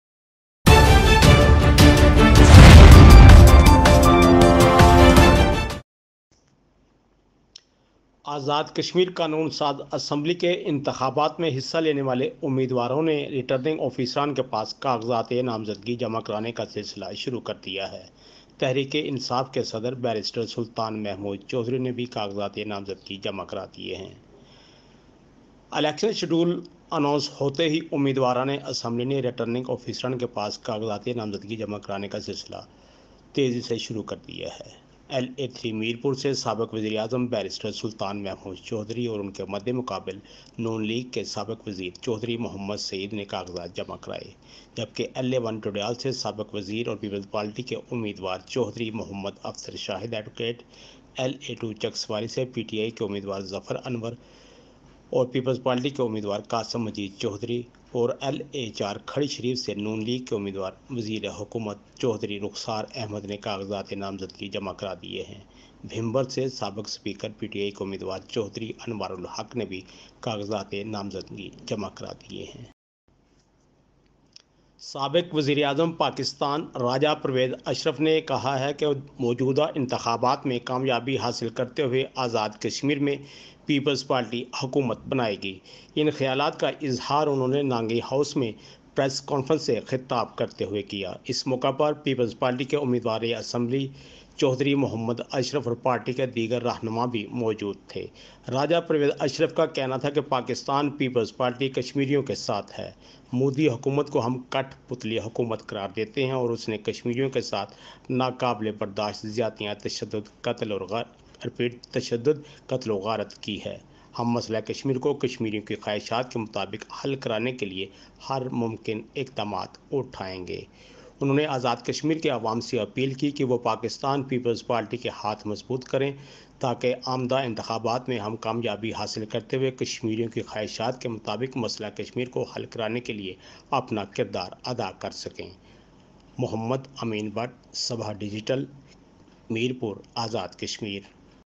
Azad Kashmir Elections Barrister Sultan & other leaders submit forms/ Raja Pervez Ashraf media Talk
آزاد کشمیر انتخابی دنگل/کاغذات نامزدگی جمع کرانے کا سلسلہ جاری
بیرسٹرسلطان محمودچوہدری ودیگر نے کاغذات نامزدگی جمع کرادیئے
 آزاد کشمیر میں پیپلزپارٹی  حکومت بنائے گی، راجہ پرویزاشرف کا دعوی